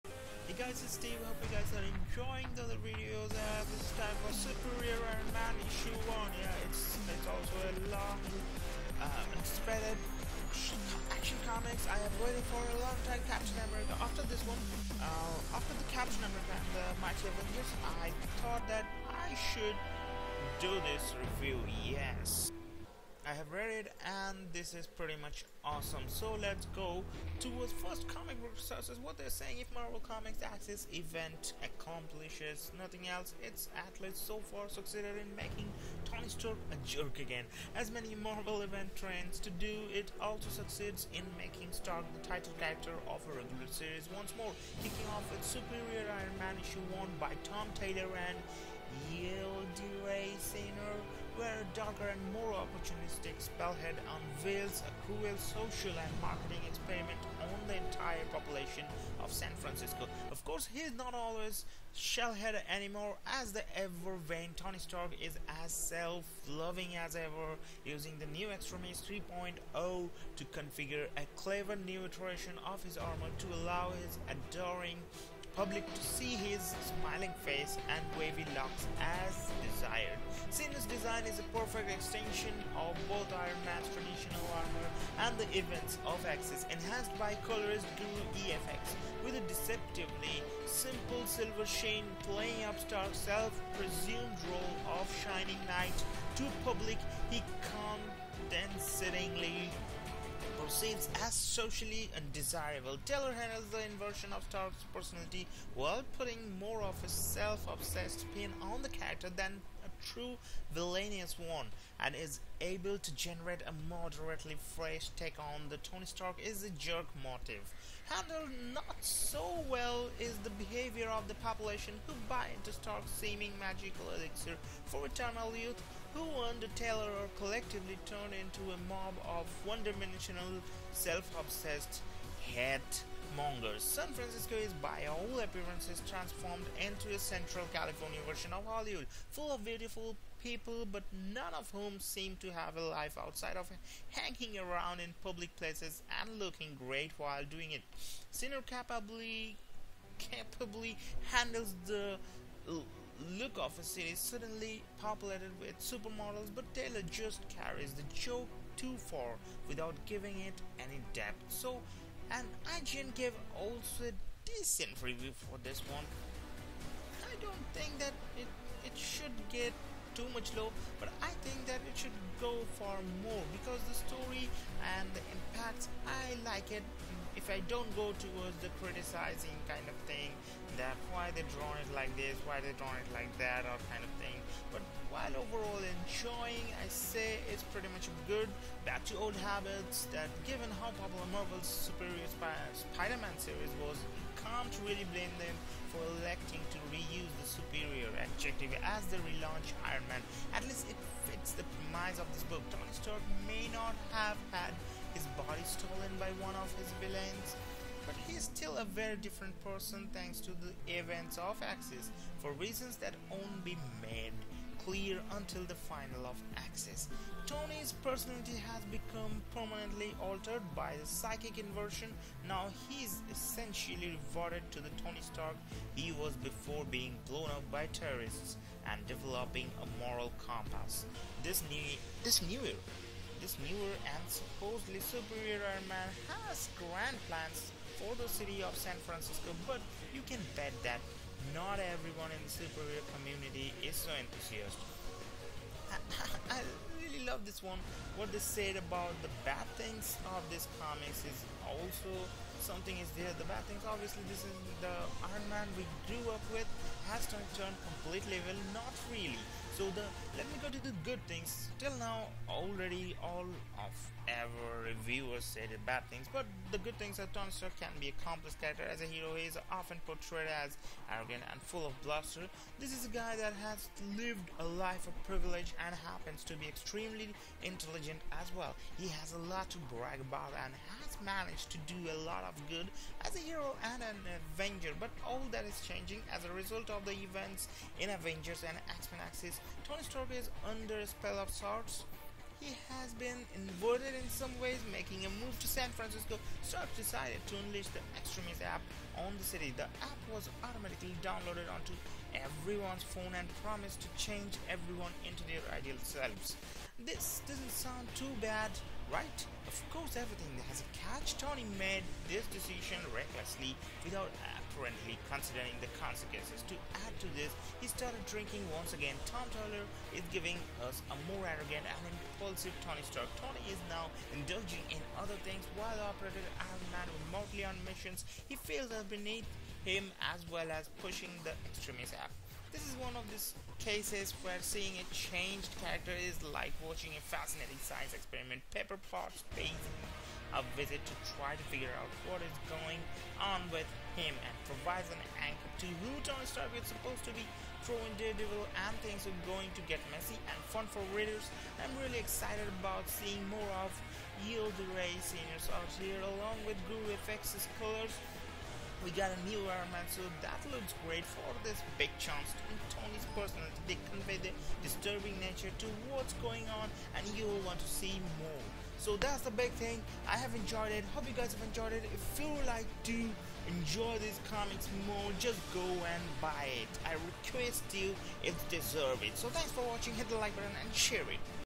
SUPERIOR IRON MAN (2014) #1 Review
One more New Series. I know you guys will Enjoy this one.

SUPERIOR IRON MAN (2014) #1
Published: November 12, 2014
Writer: Tom Taylor
Cover Artist: Mike Choi
BE SUPERIOR! NEW CREATIVE TEAM TOM (INJUSTICE: GODS AMONG US/EARTH 2) Taylor AND YILDIRAY (SUPERGIRL) ÇINAR START AN ALL-NEW SERIES SPINNING OUT OF AXIS! • How much would you pay for perfection, beauty…immortality? TONY STARK is going to find out. • The AXIS EFFECT has changed IRON MAN • Now HE’S going to change the WORLD…at a terrible cost.